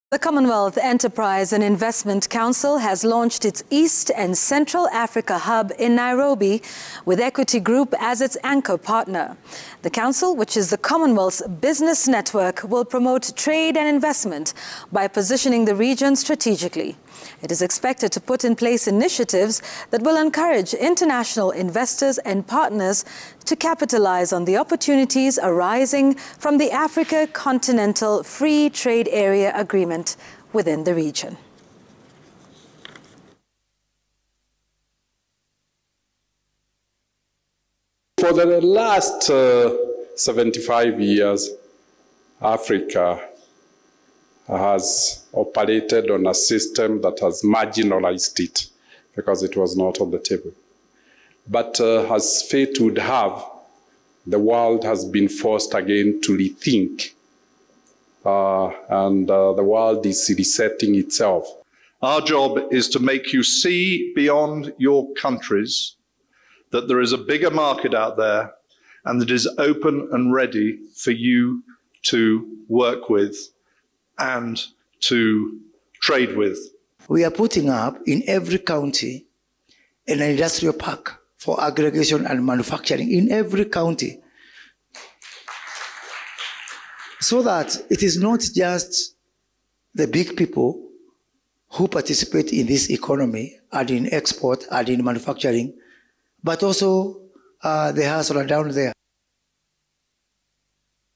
Commonwealth Enterprise and Investment Council launches its East and Central Africa hub in Nairobi
The Commonwealth Enterprise and Investment Council has launched its East and Central Africa hub in Nairobi with Equity Group as its anchor partner. The council which is the Commonwealth’s business network will promote trade and investment by positioning the region strategically. It is expected to put in place initiatives that will encourage international investors and partners to capitalize on the opportunities arising from the Africa Continental Free Trade Area Agreement within the region.